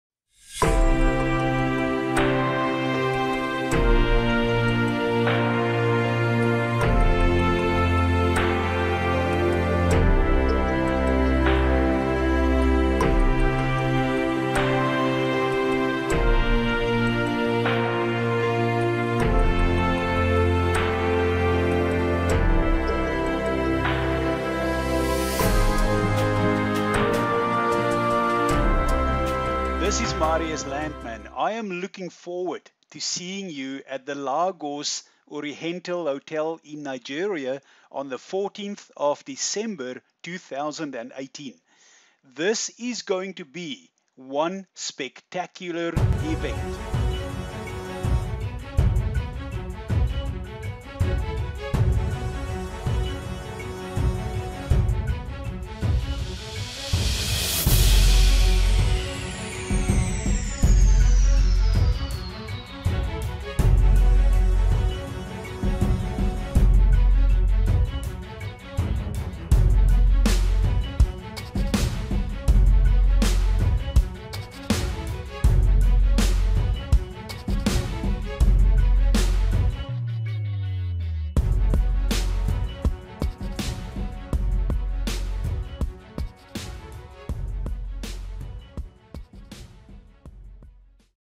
Bitcoin goes to West Africa 14 December 2018
If you are in the area join as at the Oriental Hotel in Lagos, Nigeria to see the biggest transfer of wealth - Bitcoin for the people!